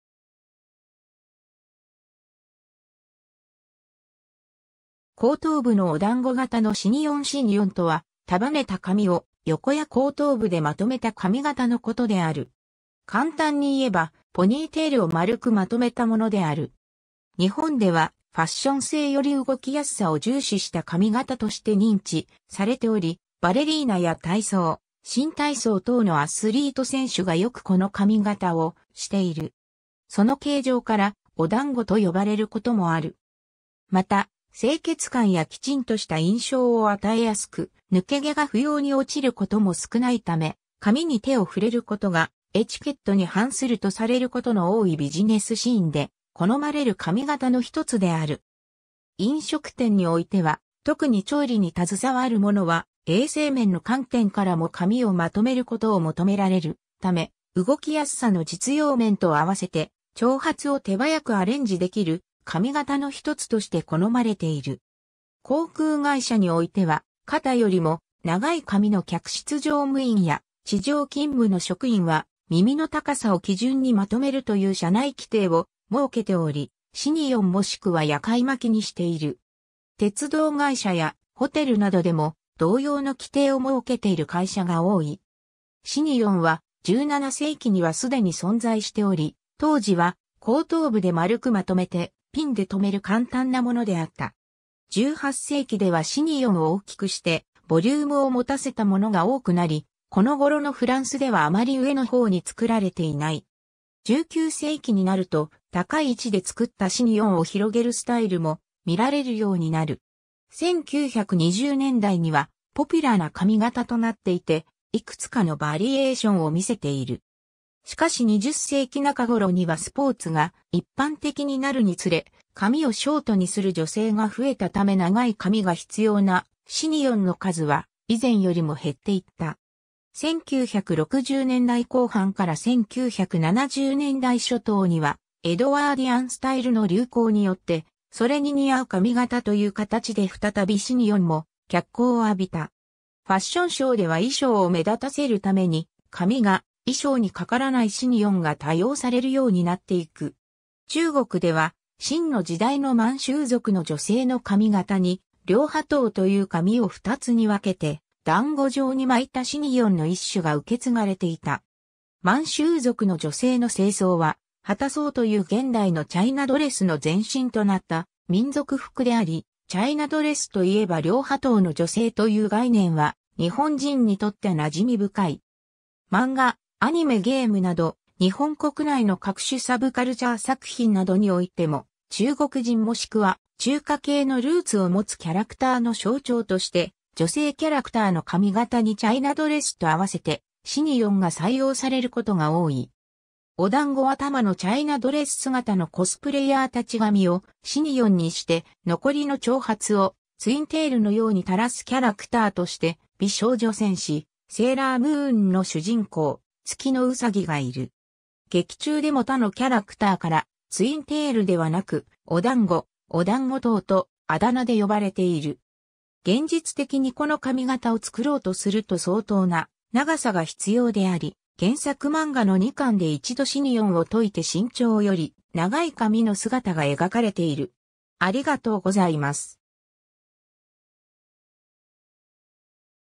シニヨン
後頭部のお団子型のシニヨン シニヨン（フランス語: chignon、シニョンとも）とは、束ねた髪を横や後頭部でまとめた髪型のことである。
簡単に言えば、ポニーテールを丸くまとめたものである。
 日本では、ファッション性より動きやすさを重視した髪型として認知されており、バレリーナや体操、新体操等のアスリート選手がよくこの髪型をしている。
その形状から「お団子（頭）」と呼ばれる事もある。
 また、清潔感やきちんとした印象を与えやすく、抜け毛が不要に落ちることも少ないため、髪に手を触れることがエチケットに反するとされることの多いビジネスシーンで好まれる髪型の一つである。
 飲食店においては、特に調理に携わる者は衛生面の観点からも髪をまとめることを求められるため、動きやすさの実用面と併せて、長髪を手早くアレンジ出来る髪型の一つとして好まれている。
 航空会社においては、肩よりも長い髪の客室乗務員や地上勤務の職員は、耳の高さを基準にまとめるという社内規定を設けており、シニヨンもしくは夜会巻きにしている。
鉄道会社やホテルなどでも同様の規定を設けている会社が多い。
 シニヨンは、17世紀にはすでに存在しており、当時は後頭部で丸くまとめてピンでとめる簡単なものであった。
 18世紀ではシニヨンを大きくしてボリュームを持たせたものが多くなり、この頃のフランスではあまり上の方に作られていない。
 19世紀になると、高い位置で作ったシニヨンを広げるスタイルも見られるようになる。
 1920年代にはポピュラーな髪型となっていて、いくつかのバリエーションをみせている。
しかし20世紀中頃にはスポーツが一般的になるにつれ、髪をショートにする女性が増えたため長い髪が必要なシニヨンの数は以前よりも減っていった。
 1960年代後半から1970年代初頭には、エドワーディアンスタイル（胸を強調し、ウエストを絞って、ふわっと広がったスカートのドレス）の流行によって、それに似合う髪型という形で再びシニヨンも脚光を浴びた。
 ファッションショーでは衣装を目立たせるために、髪が衣装にかからないシニヨンが多用されるようになっていく。
 中国では、清の時代の満洲族の女性の髪形に、両把頭という髪を二つに分けて団子状に巻いたシニヨンの一種が受継がれていた。
 満州族の女性の正装は、旗装という現代のチャイナドレスの前身となった民族服であり、「チャイナドレスといえば両把頭の女性」という概念は日本人にとって馴染み深い。
 漫画・アニメ・ゲームなど、日本国内の各種サブカルチャー作品などにおいても、中国人もしくは中華系のルーツを持つキャラクターの象徴として、女性キャラクターの髪型にチャイナドレスとあわせてシニヨンが採用されることが多い。
(らんま1/2のシャンプー、ストリートファイターの春麗等) お団子頭のチャイナドレス姿のコスプレイヤー達 髪をシニヨンにして、残りの長髪をツインテールのように垂らすキャラクターとして『美少女戦士セーラームーン』の主人公・月野うさぎがいる。
劇中でも他のキャラクターからツインテールではなく「お団子」「お団子頭」とあだ名で呼ばれている。
現実的にこの髪型を作ろうとすると相当な長さが必要であり、原作漫画の2巻で一度シニヨンを解いて身長より長い髪の姿が描かれている。